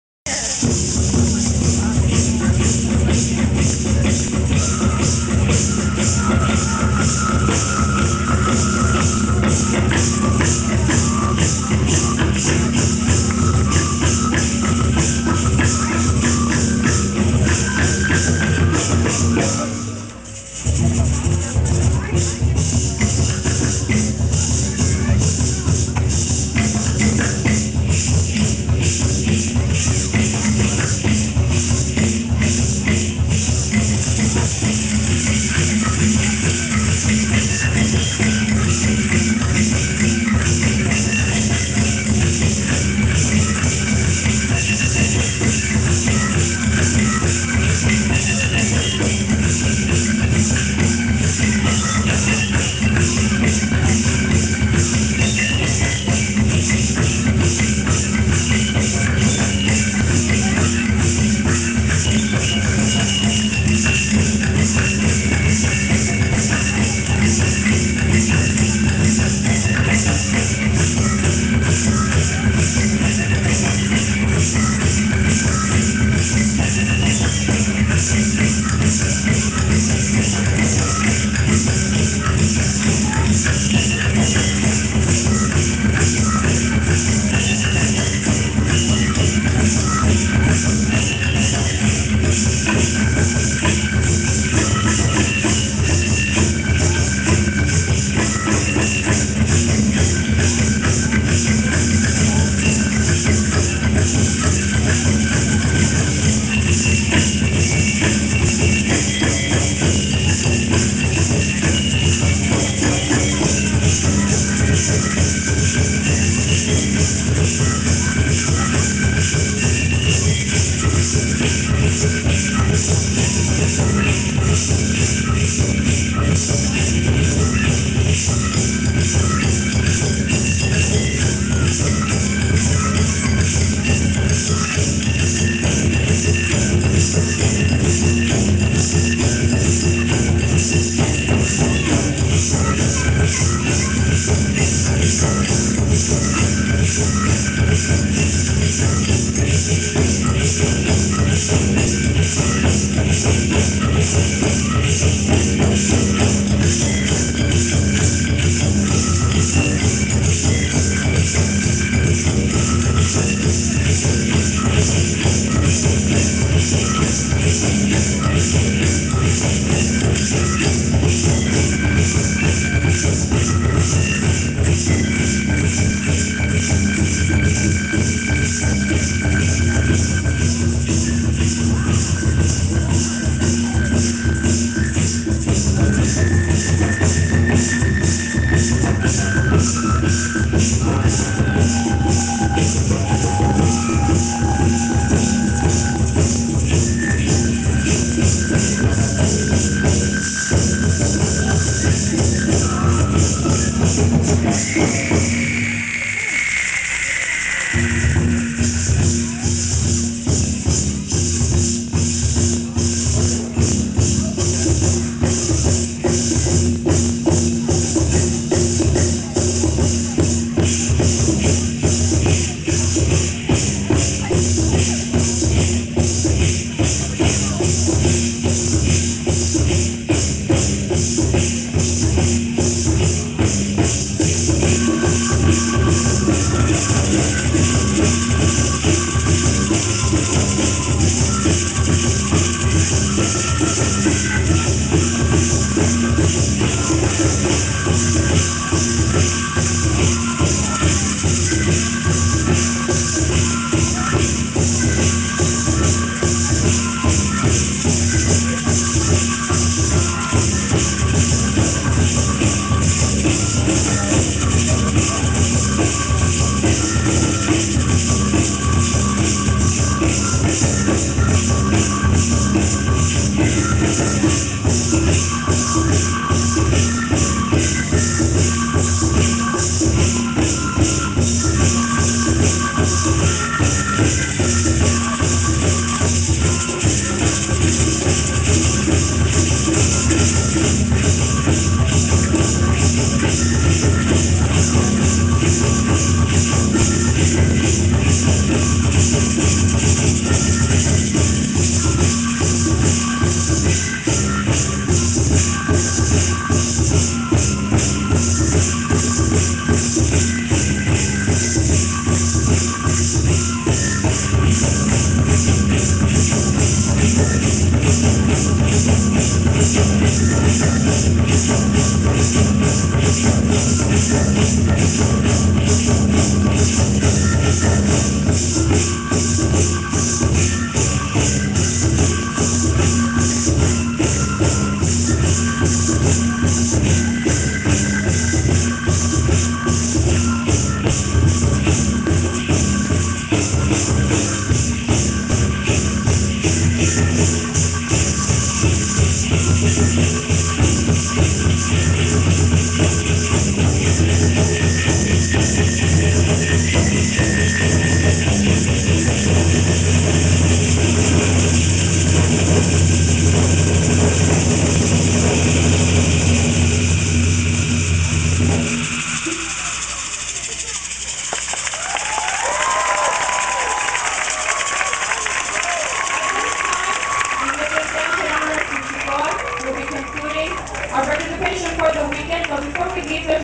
REDHAWK Native American Arts Council POW WOW 3
Invited guests: Aztec Dancers perform a ritual fire dance!